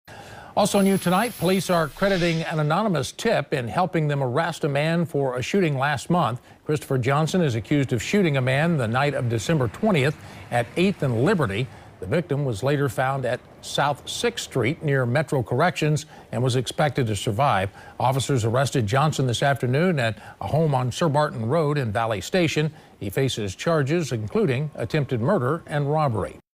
32-year-old charged with attempted murder in connection to downtown Louisville shooting
32-year-old charged with attempted murder in connection to downtown Louisville shooting